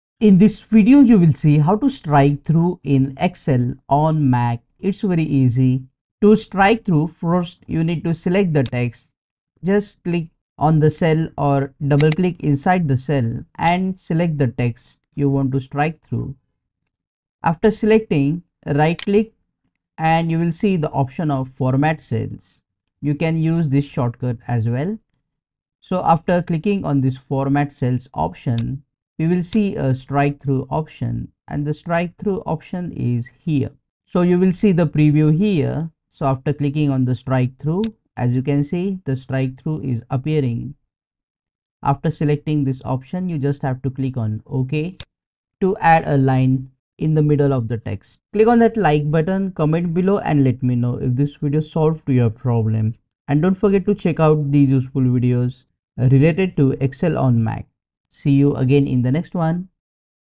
How to StrikeThrough in Excel [ MAC ]
This video show How to Strikethrough in Excel MAC OS on MacBook. To cross out or put line through text select the text in Excel Sheet using keyboard or mouse and then right-click and click on the Format cells option. Select the strikethrough option and click ok!